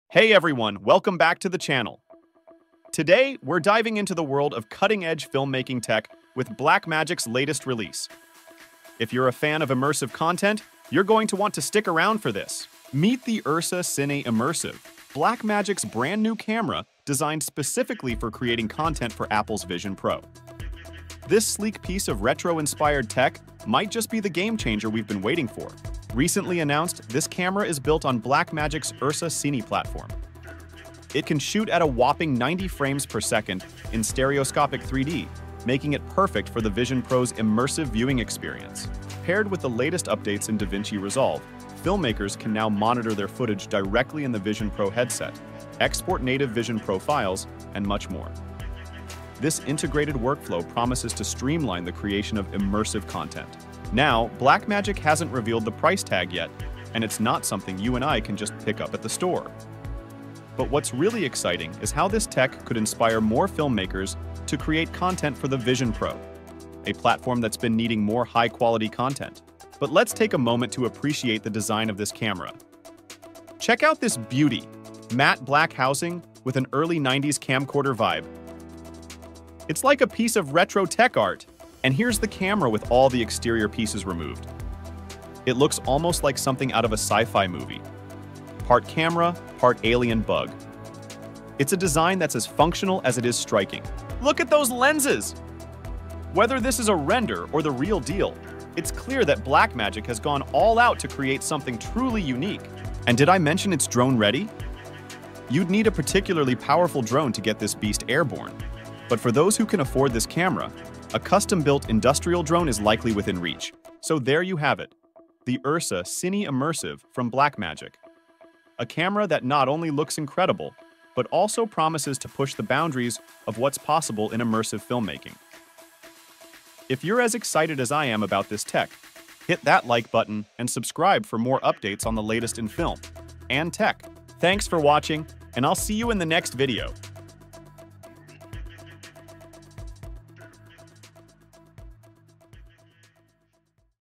Introducing Blackmagic's URSA Cine Immersive: The Future of Vision Pro Content Creation!
Discover Blackmagic's latest innovation – the URSA Cine Immersive camera! Designed to revolutionize content creation for Apple's Vision Pro, this retro-inspired, high-tech camera shoots stunning 90fps stereoscopic 3D video. Paired with the latest updates in DaVinci Resolve, it offers a seamless workflow for filmmakers. Join us as we dive into the features, design, and potential impact of this groundbreaking camera. Don't miss out – watch now to learn more!